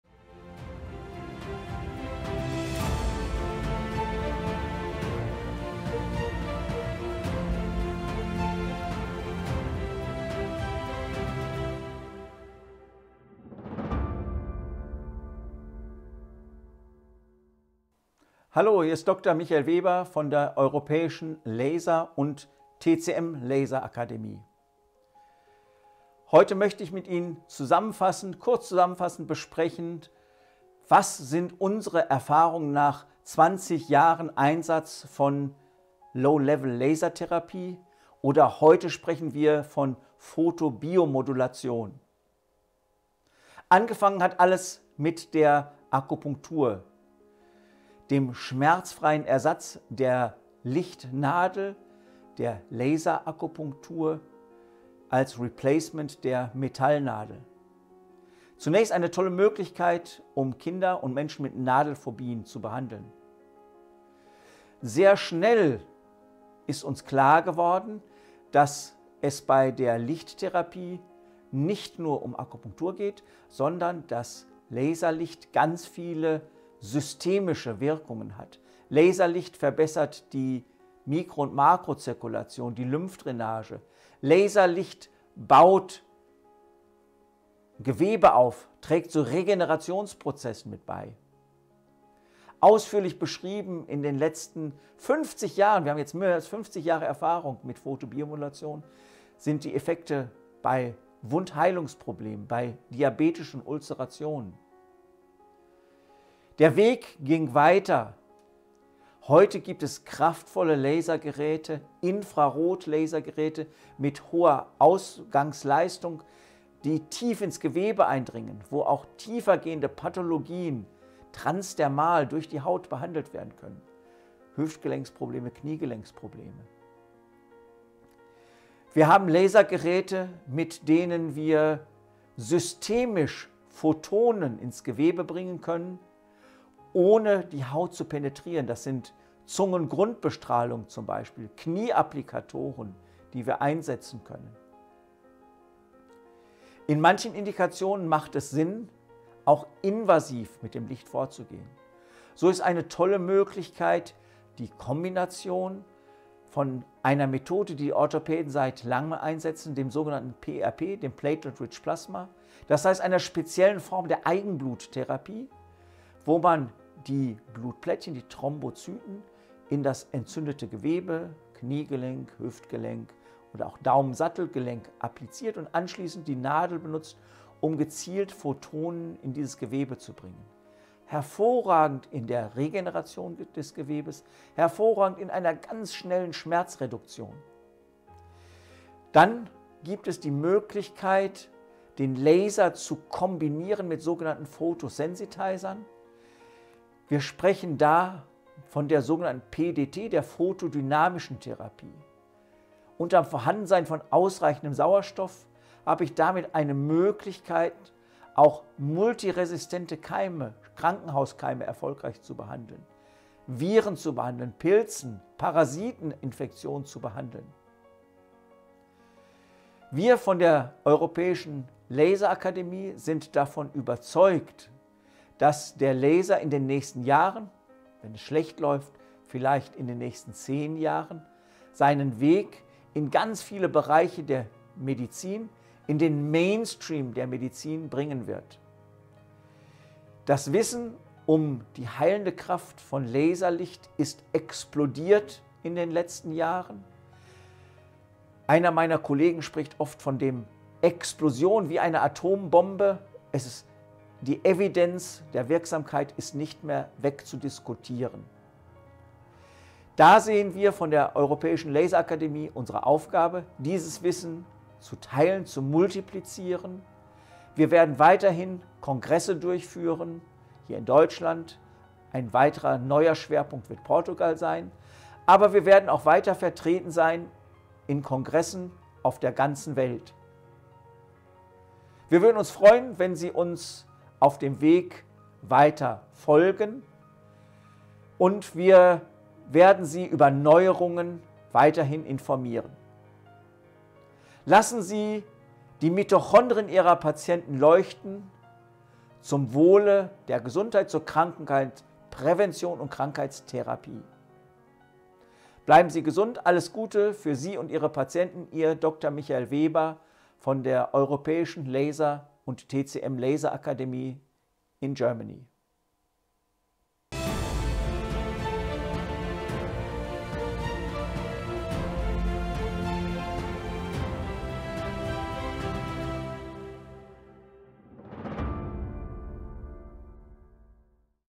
Willkommen bei der European TCM-Laser Akademie
Wir freuen uns, Sie auf der Homepage der „European TCM-Laser Academy“ begrüßen zu dürfen.
Das Ziel der Akademie ist es, das Wissen um die faszinierenden Möglichkeiten der Laserakupunktur an alle interessierten Therapeuten aus dem Bereich der Traditionellen Chinesischen Medizin zu vermitteln. In unseren Kursen und Seminaren liefern wir vielerlei nützliche Anregungen für den Einsatz der „LaserLichtTherapie“ in der täglichen Praxis. Darüber hinaus ermöglichen wir einen internationalen wissenschaftlichen Austausch über die neuen Erkenntnisse der „Laserakupunktur“.